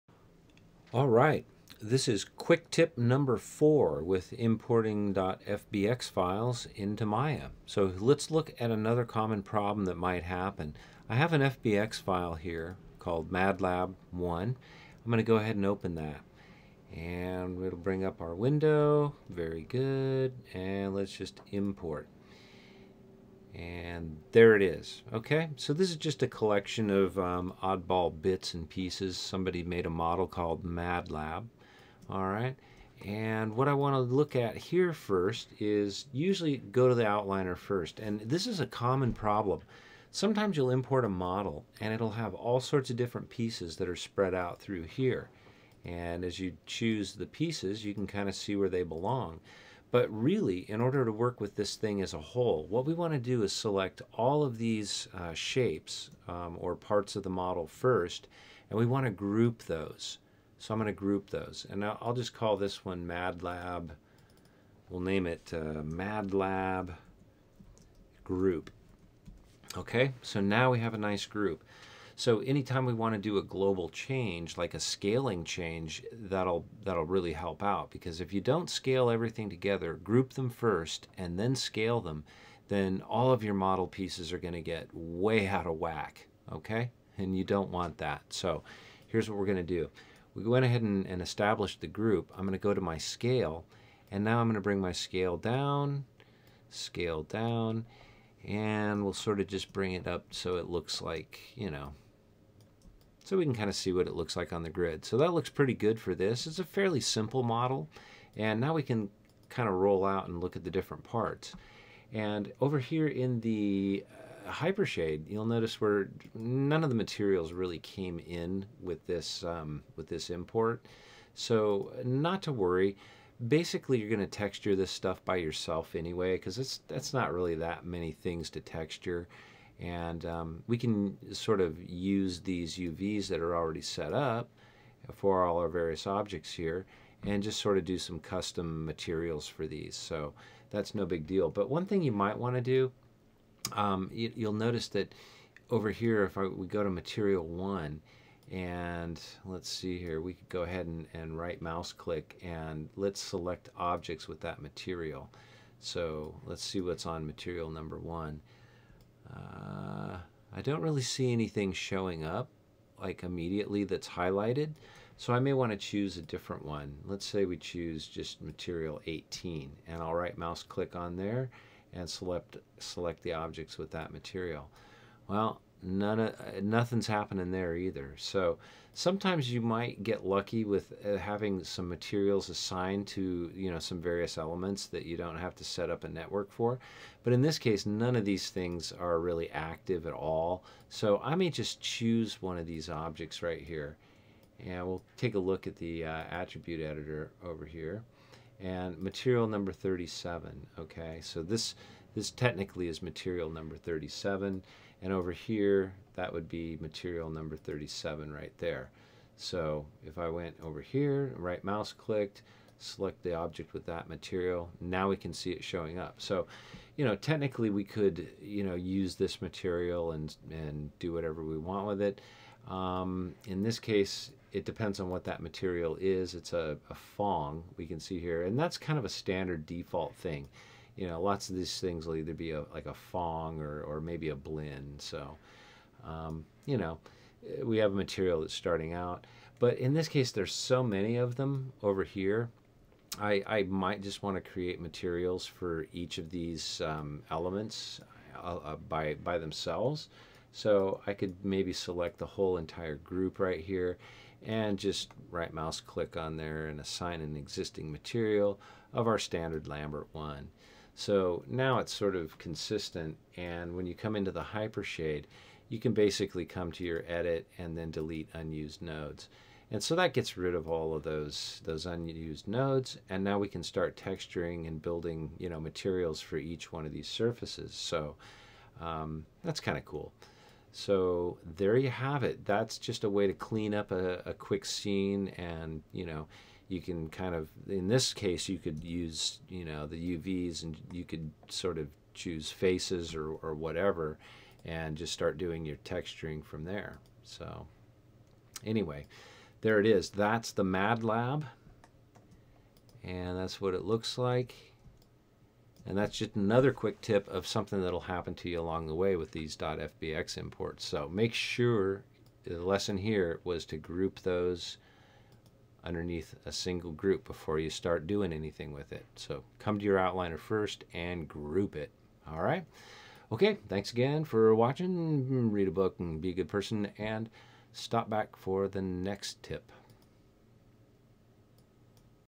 Maya .3ds Model to Maya FBX Conversion Quicktip_Proper Grouping Tutorial by Stuart Christensen
A quicktip tutorial by Stuart Christensen on how to deal with proper grouping on model parts that were converted from a .3ds file to .FBX for use in Maya.